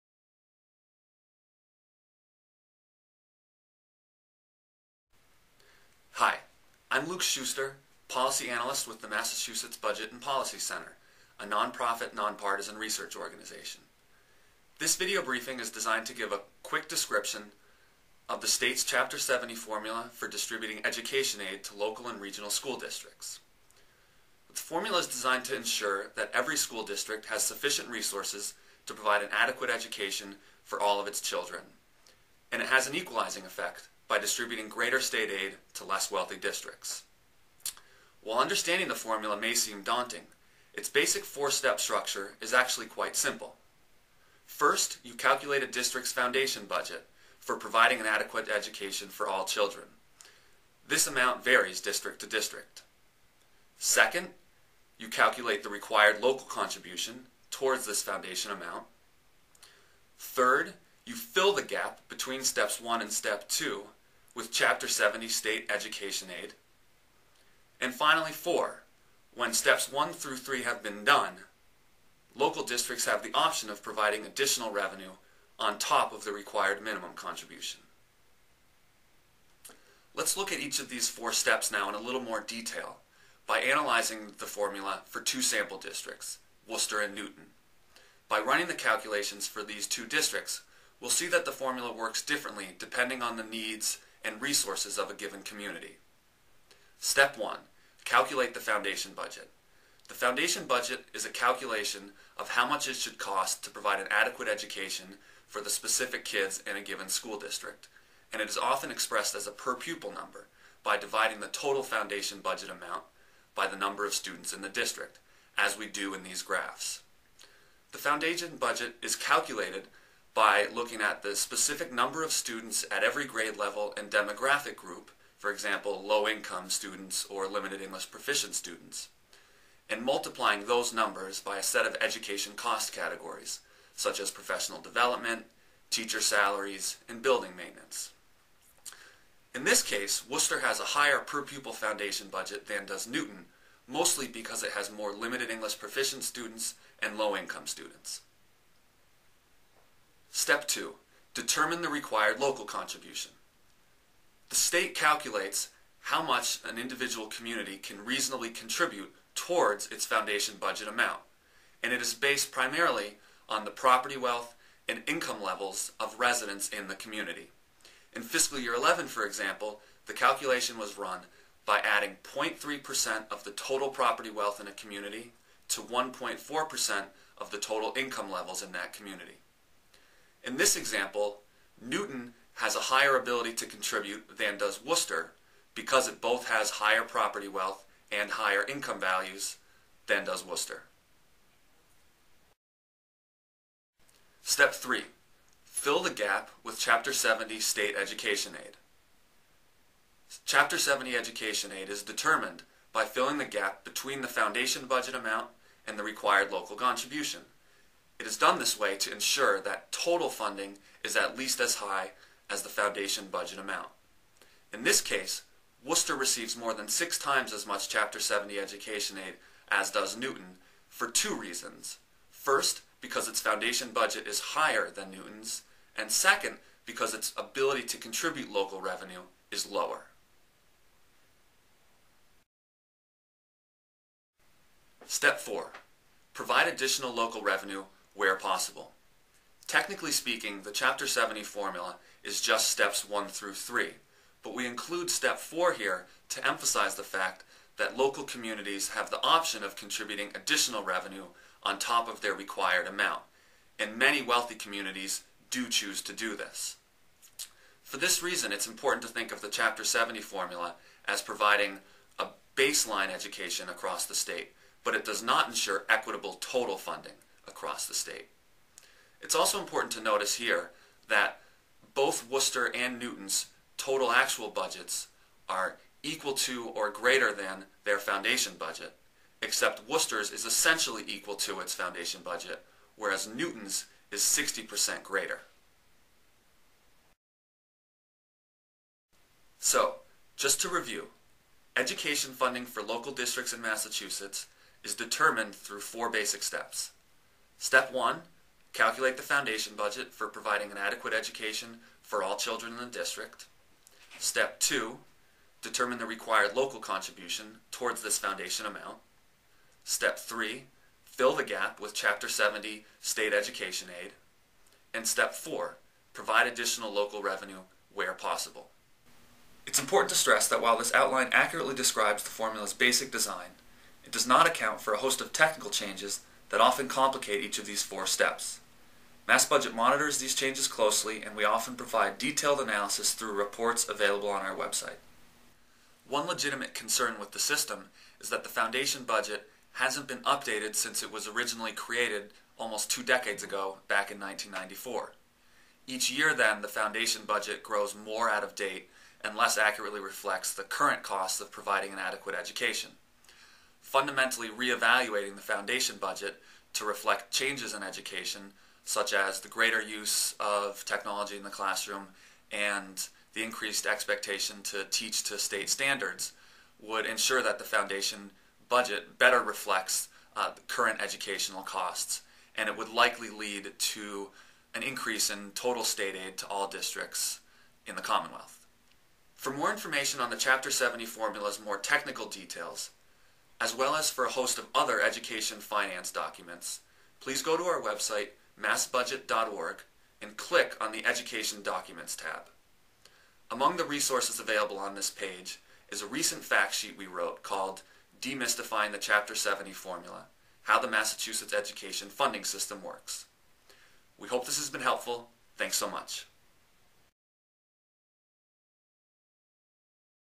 Demystifying the Chapter 70 Formula — How the Massachusetts Education Funding System Works
A MassBudget video briefing on the Chapter 70 formula for distributing education aid to local and regional school districts in Massachusetts. While understanding the formula may seem daunting, its basic structure is actually quite simple, as demonstrated by the four steps outlined in this video.